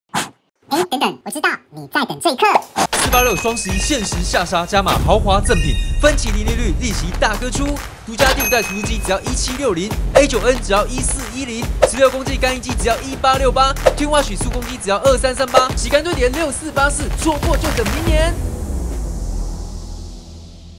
486 雙11家電最殺優惠還可以抽機票，送你出國去！
486雙11來了，搭配無卡分期滿額抽「法國、日本雙人機票」。

《Mr486》APP推出最新二手家電市集，想怎麼買、怎麼賣隨便你，還不用被抽手續費；在486買過家電的朋友更要立刻下載，所有問題線上真人客服回覆，還能跟其他人一起討論交流。

※11/6前下載註冊就能參加抽獎，連續八週，週週抽總價超過百萬的黃金和熱門家電！
本節目由486團購製作
好吃好玩好物
盡在486團購
更多486系列頻道推薦：